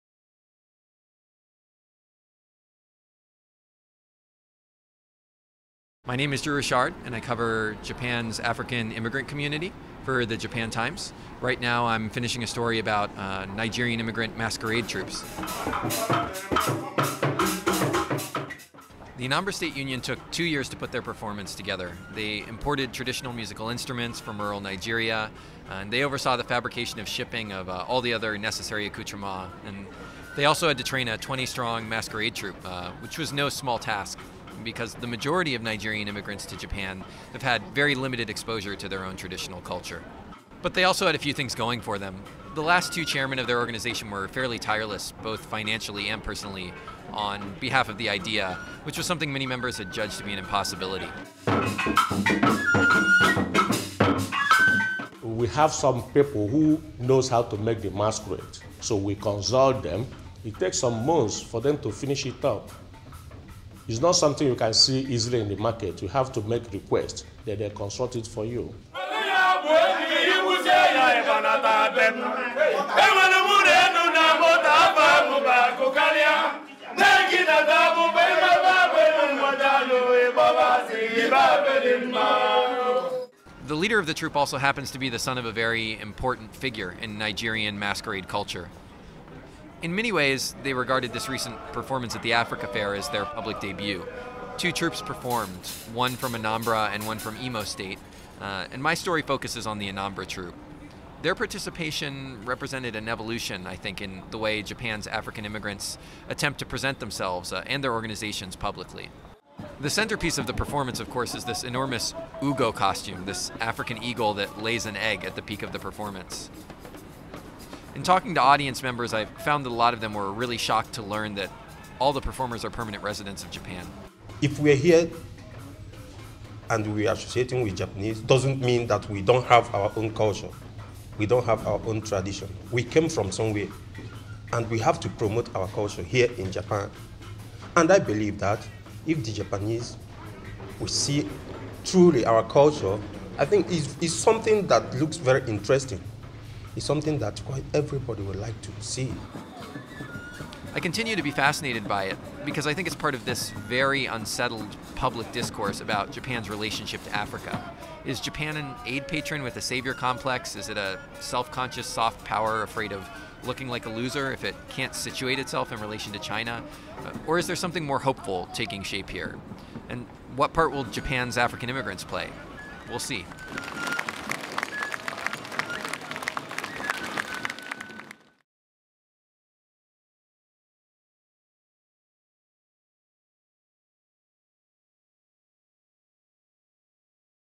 Japan's Nigerians see symbol of change in masquerade
The local Nigerian expat community groups are bringing the colorful tradition of masquerade to their adopted home for the first time. Video produced by the Shingetsu News Agency for The Japan Times.